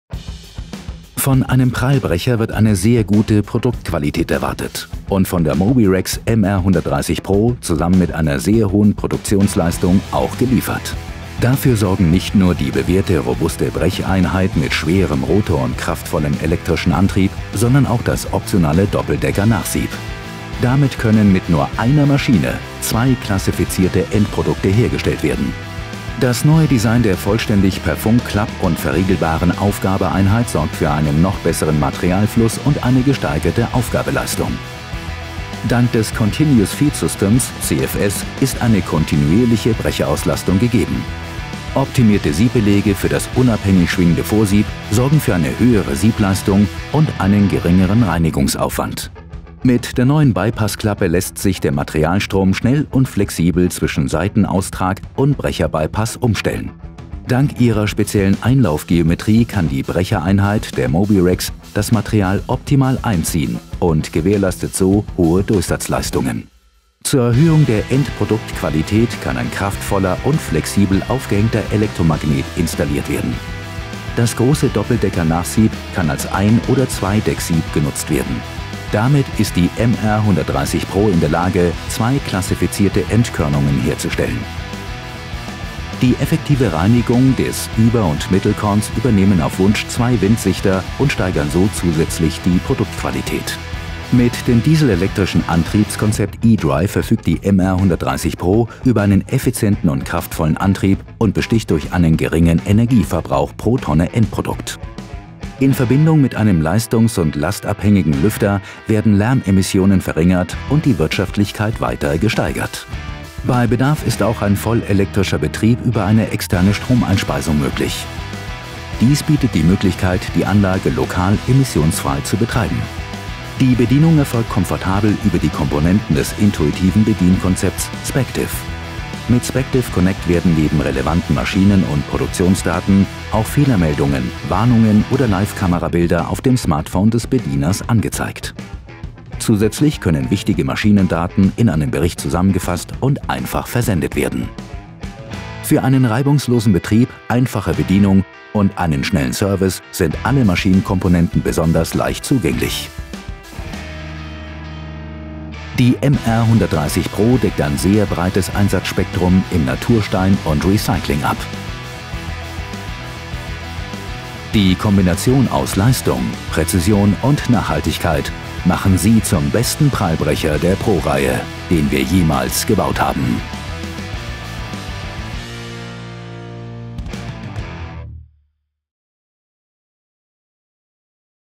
MOBIREX MR130(i) PRO - Nachhaltigkeit trifft Powerplay | mobile Brechanlage
Von einem Prallbrecher wird sehr gute Produktqualität erwartet – und von der fortschrittlichen KLEEMANN MOBIREX MR 130(i) PRO inklusive einer sehr hohen Produktionsleistung auch geliefert.

Dafür sorgt nicht nur die bewährte robuste Brecheinheit mit ihren zwei Prallschwingen und vielseitigen Schlagleisten-Optionen, sondern auch die 8,4 m2 große optionale Doppeldecker-Siebeinheit.

Damit ist es ohne den Einsatz einer zusätzlichen Siebanlage mit nur einer Maschine möglich, zwei klassifizierte Endkörnungen herzustellen. Die Anlage überzeugt mit ihrer Brecheinheit mit schwerem Rotor und kraftvollem elektrischem 250-kW-Antrieb, was zusammen mit der effizienten Vorabsiebung für eine sehr hohe, stabile Durchsatzleistung sorgt. Dank des Continuous Feed Systems (CFS) und der optimierten Siebeinheit erzielt die MOBIREX MR 130(i) PRO eine konstant hohe Durchsatzleistung und Produktqualität. Das intuitive SPECTIVE Bedienkonzept ermöglicht eine einfache Handhabung,.

Entdecken Sie, wie der KLEEMANN MOBIREX MR 130(i) PRO Ihre Gesteinsverarbeitung optimiert.